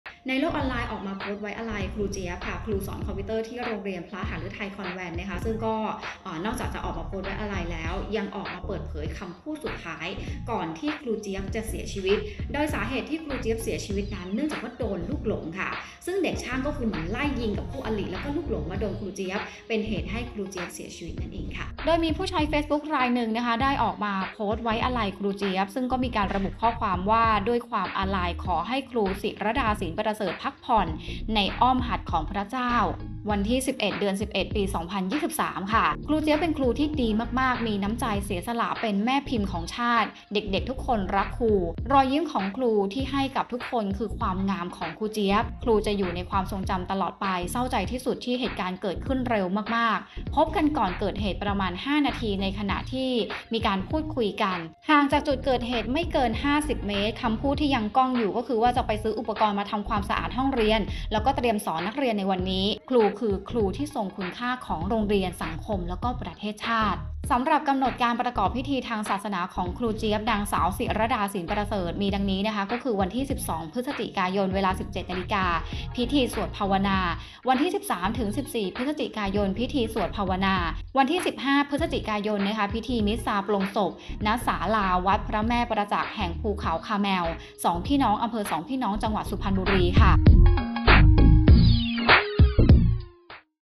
สุดเศร้า เผยคำพูดสุดท้าย "ครูเจี๊ยบ" เหยื่อลูกหลงเด็กช่าง| Thainews - ไทยนิวส์
เผยคำพูดสุดท้าย "ครูเจี๊ยบ" คุณครูโรงเรียนพระหฤทัยคอนแวนต์ เหยื่อลูกหลงกระสุนปืนเด็กช่าง ก่อนถูกยิง 5 นาที